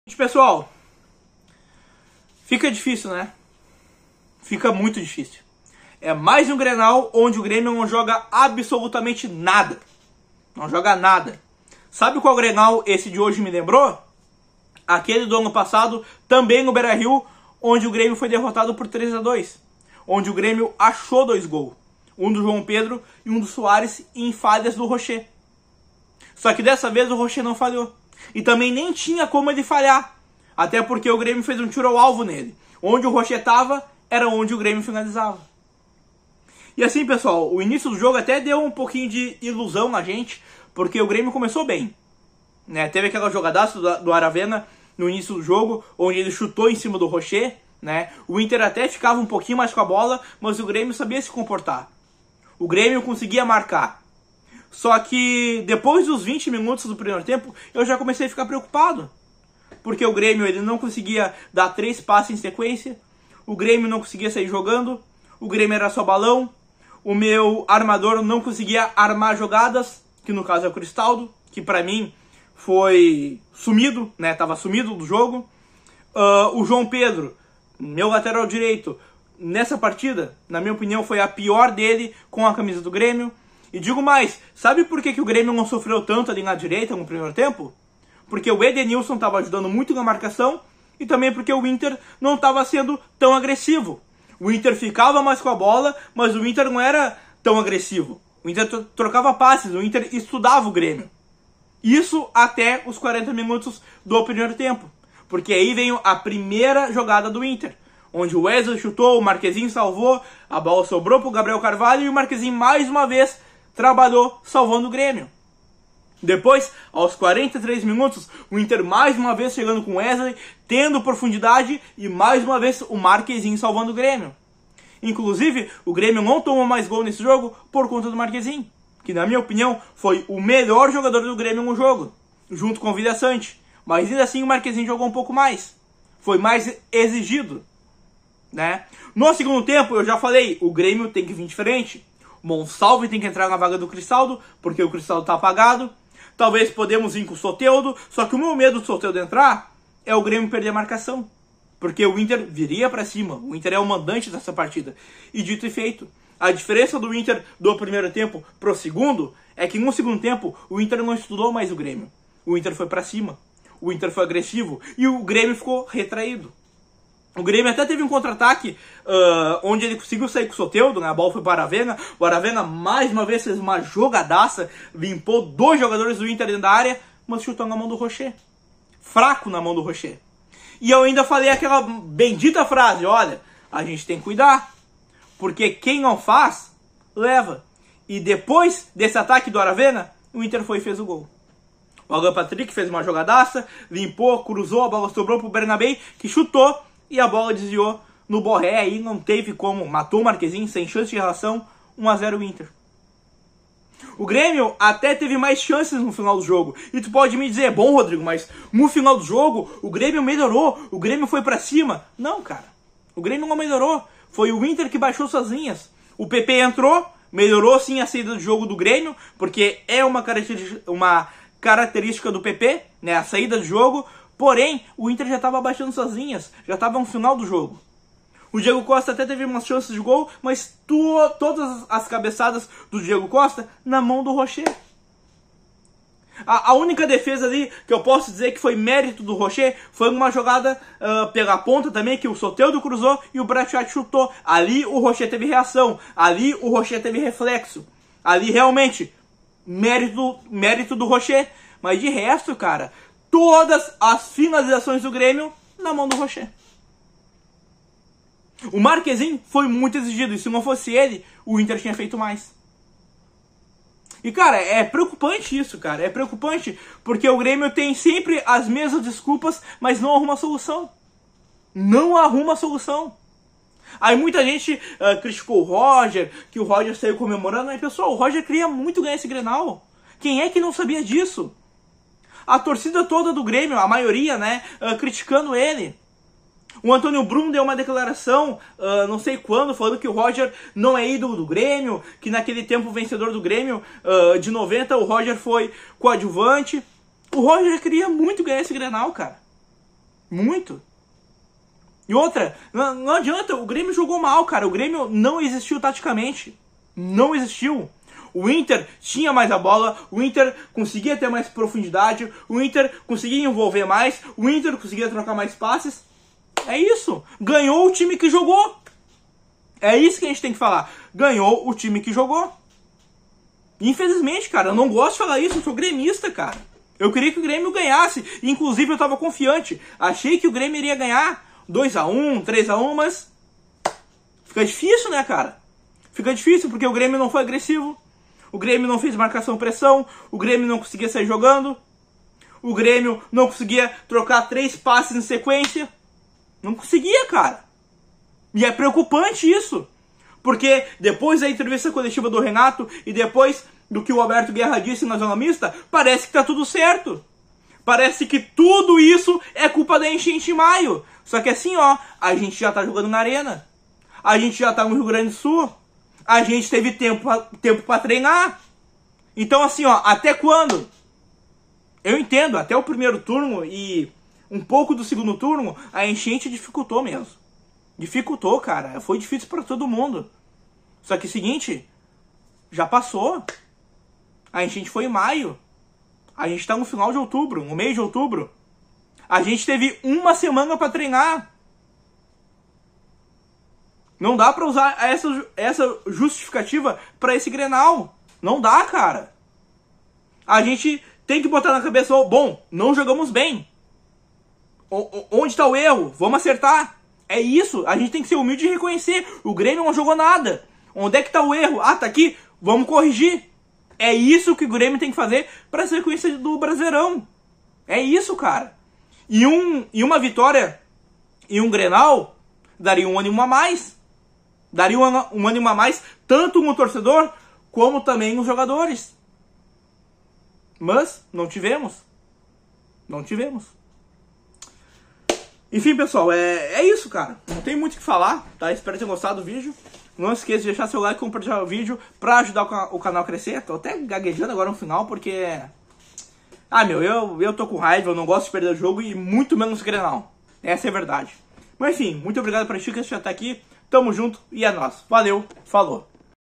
MAIS UM GREnal ONDE NÃO JOGAMOS NADA!!! | Internacional 1x0 Grêmio - Rodrigo Machado
Nesse VÍDEO vamos FALAR sobre INTER X GRÊMIO